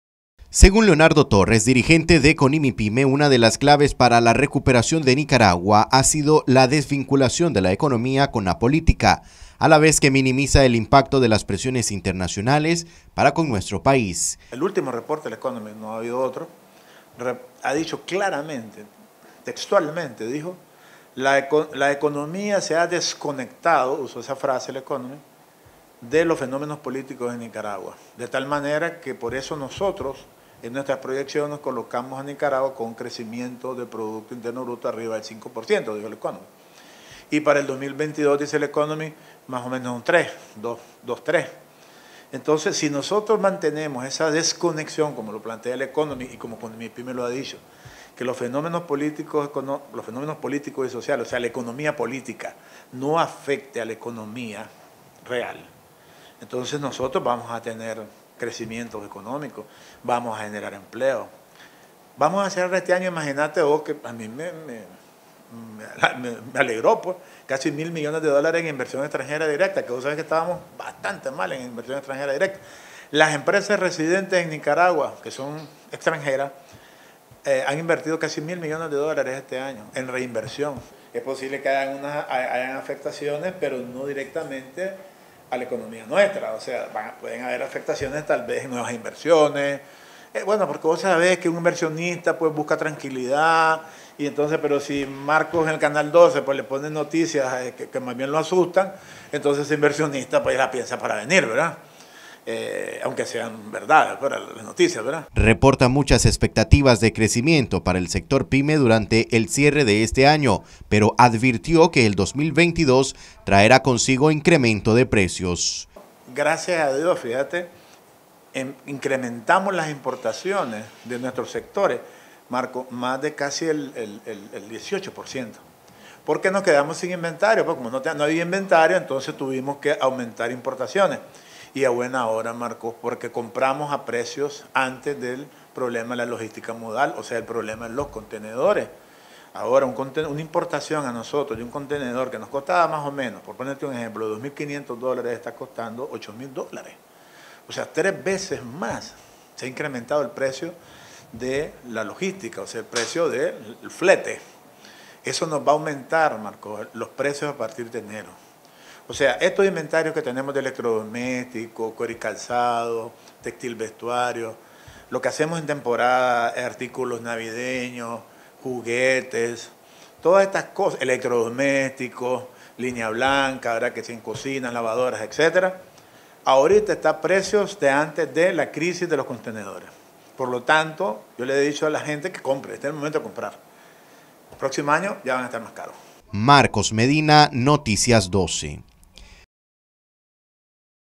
Economía se está desvinculado de la política por eso se registra recuperación
Noticias12Nicaragua Economía se está desvinculado de la política por eso se registra recuperación, según Conimipyme